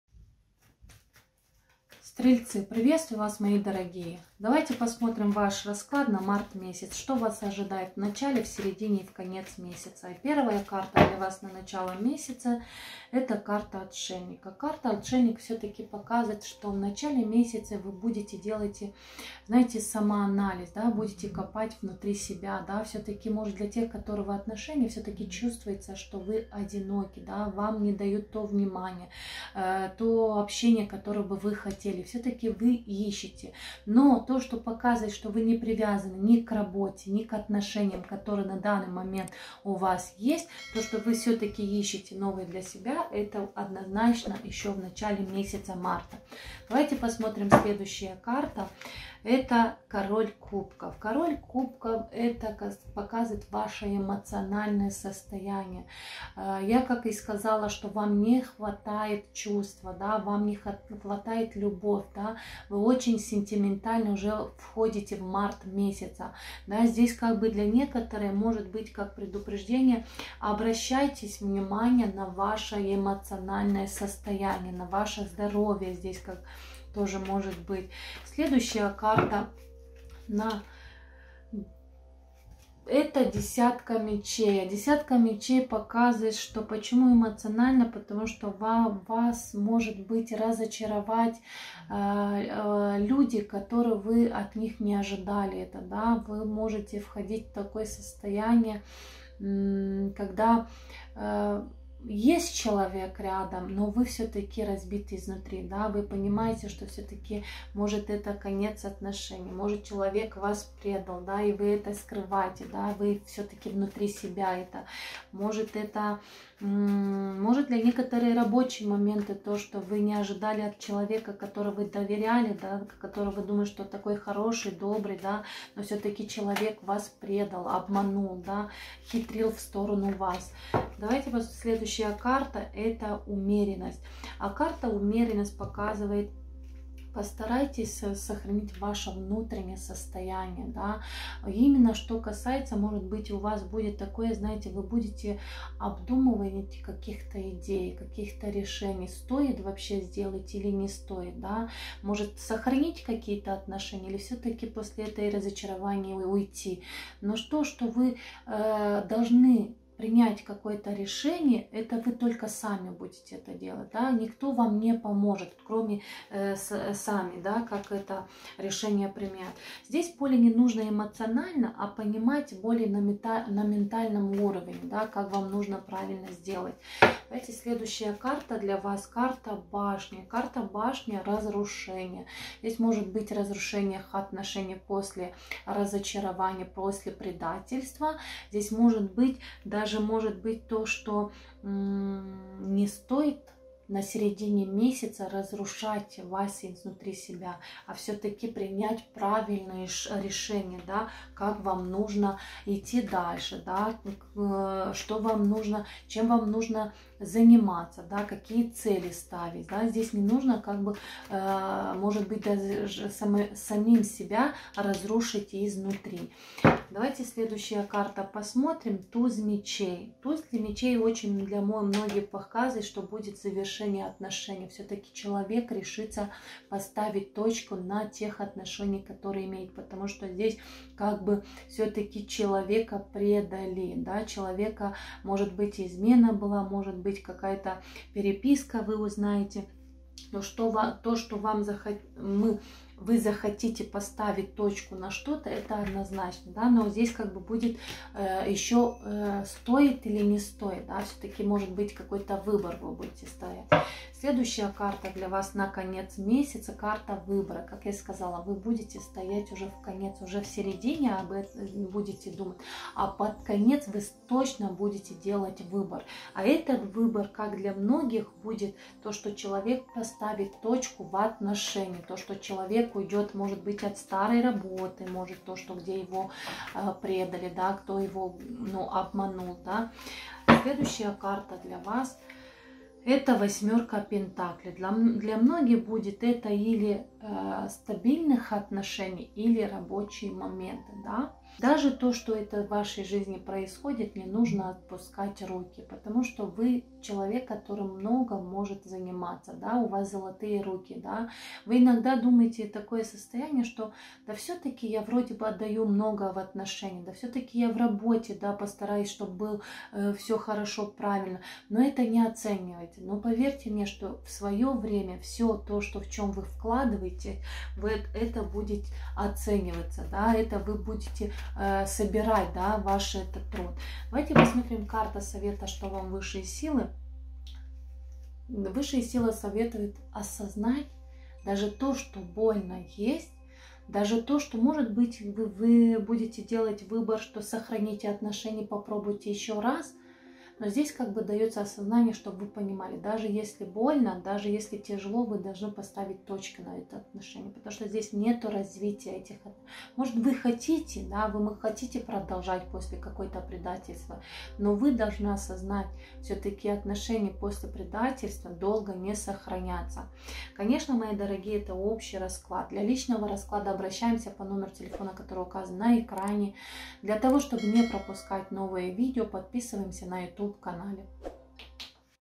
СТРЕЛЕЦ Март 2024. Таро гороскоп для СТРЕЛЬЦОВ на Март 2024 от Доины Римари
Что ждет СТРЕЛЬЦОВ в Марте 2024 года?